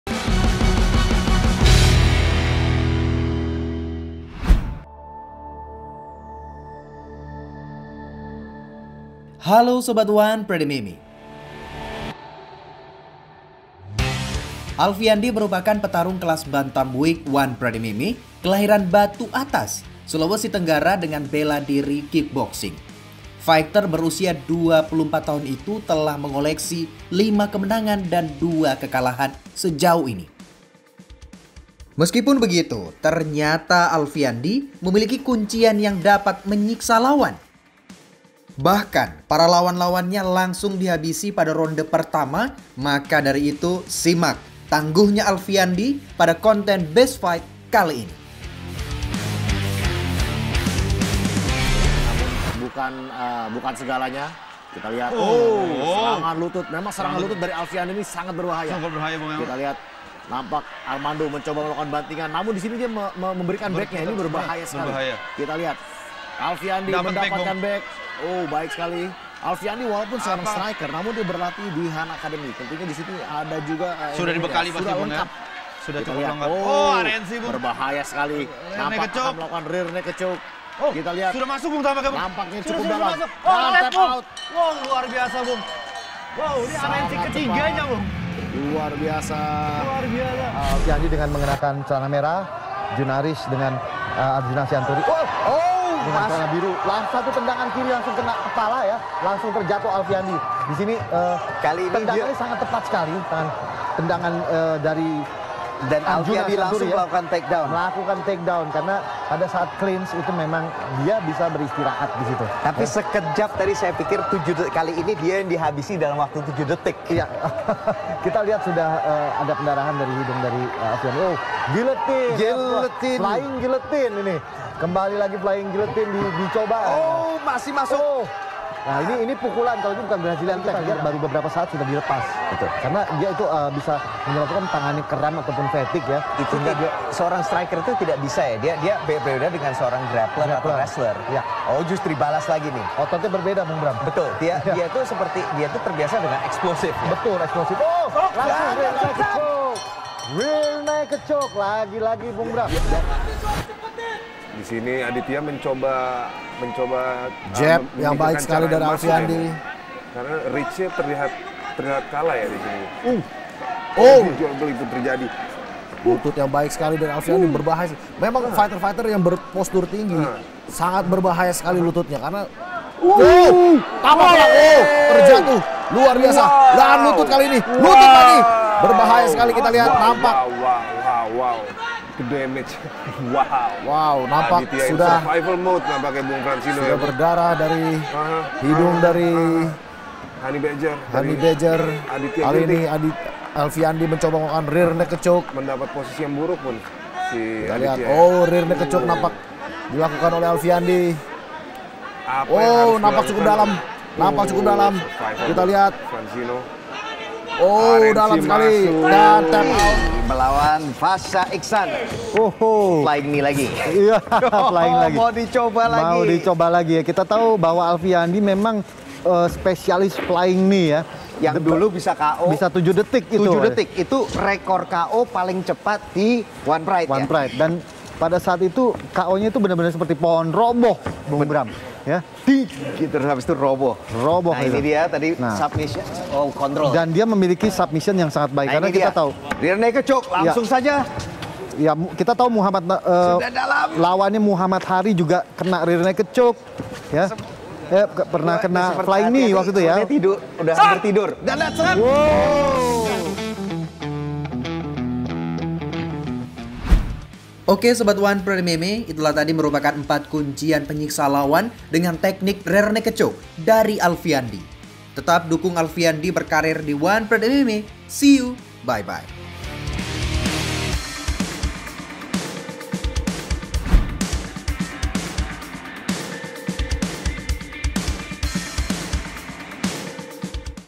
4 Kuncian Nyelekit Alfiandi, Lawan Pada Dihabisi Ronde 1! | Best Fight One Pride MMA
Jakarta, tvOnenews.com - Alfiandi merupakan petarung kelas Bantamweight One Pride MMA kelahiran Batu Atas, Sulawesi Tenggara dengan bela diri kickboxing.

Fighter berusia 24 tahun itu telah mengoleksi 5 kemenangan dan 2 kekalahan sejauh ini. Meski begitu ternyata Alfiandi memiliki kuncian yang nyelekit.

Bahkan para lawan-lawannya langsung dihabisi pada ronde 1! Maka dari itu simak tangguhnya Alfiandi pada konten Best Fight kali ini.

Tadi merupakan 4 kuncian nyelekit dengan Teknik rear naked choke dari Alfiandi. Tetap dukung Alfiandi berkarier di One Pride MMA. See you!